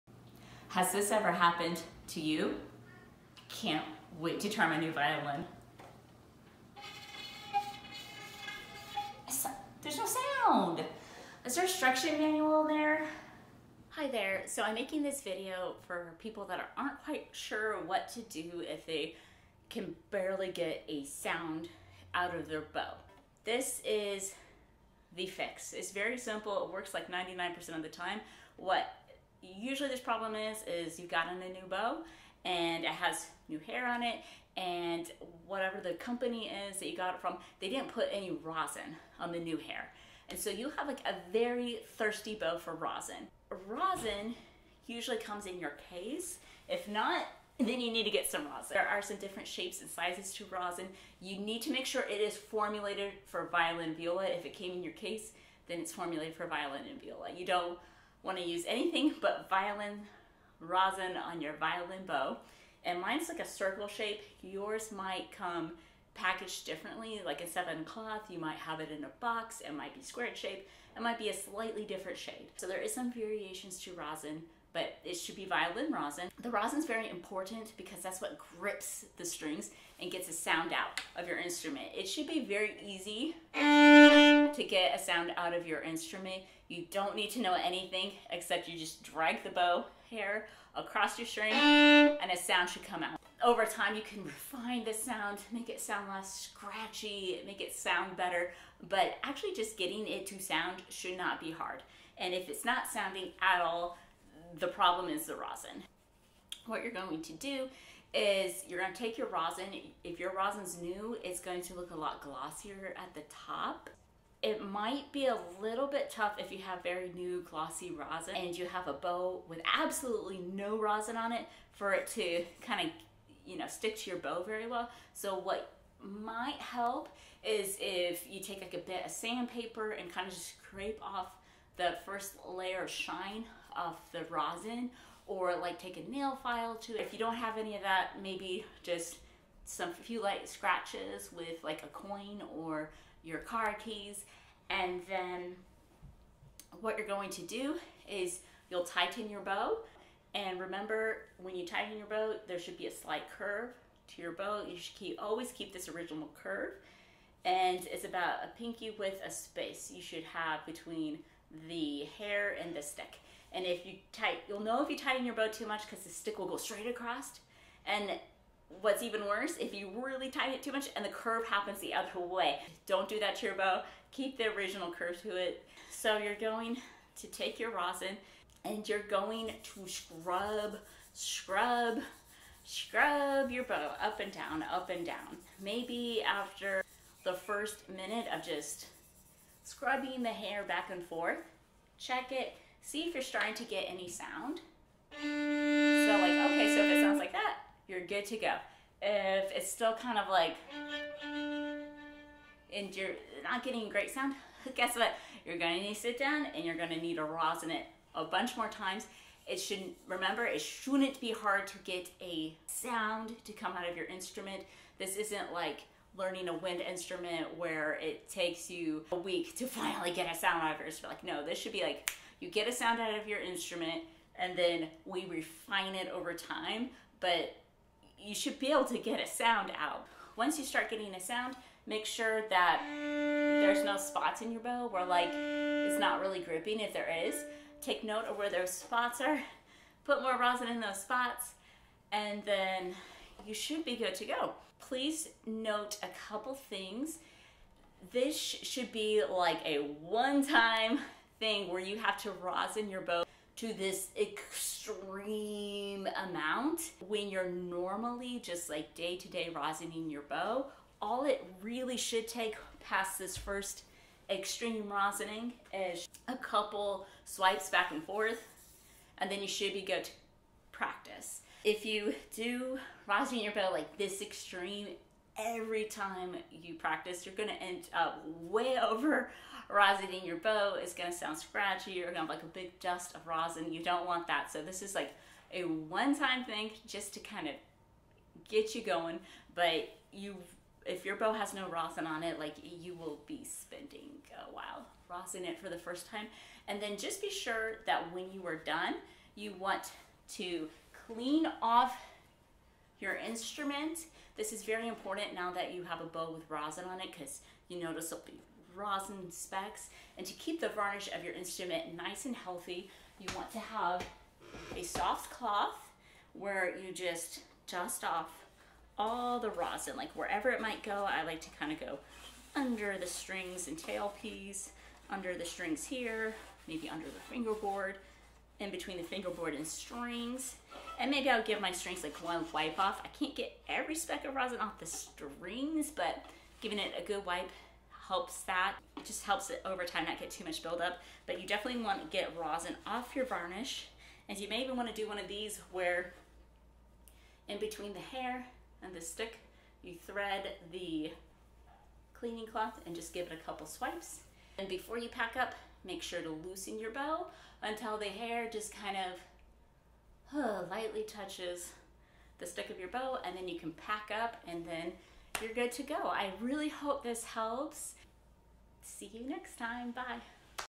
How to fix: violin bow not making sound
What to do when your violin will not make sound